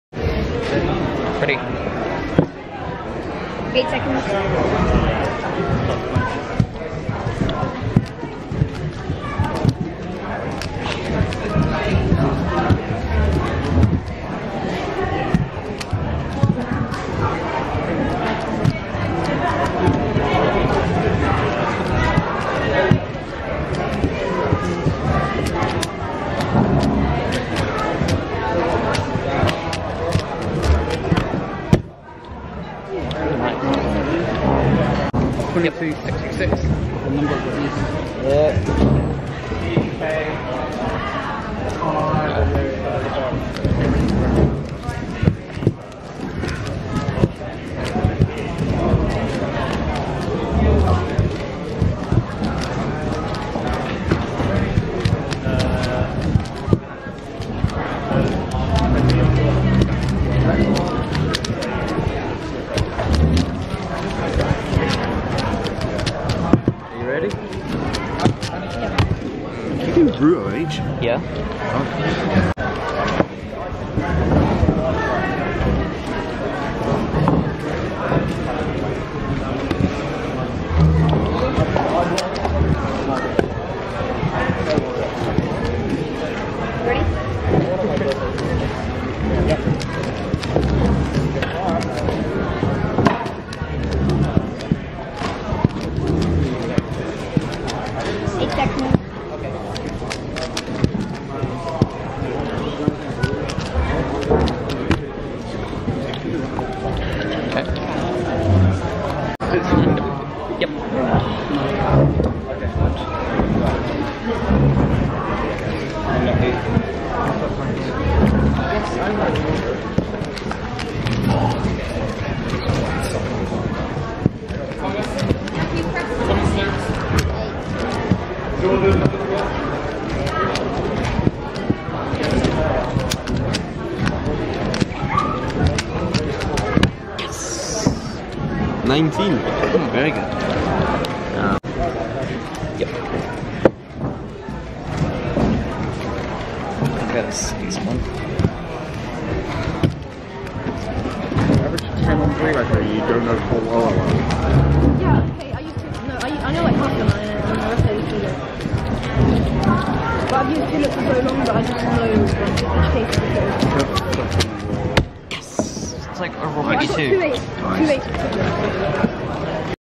22.07 Official Roux Method OH Average (15.51 Single)
Wasn't expecting this, the single is overall pb#3. Kinda annoyed that only just missed out on finals, but not the end of the world. Single is NR25 kek.

Cube: Yuxin Little Magic M

Times:
23.64, 25.87, 23.56, 19.00, 15.51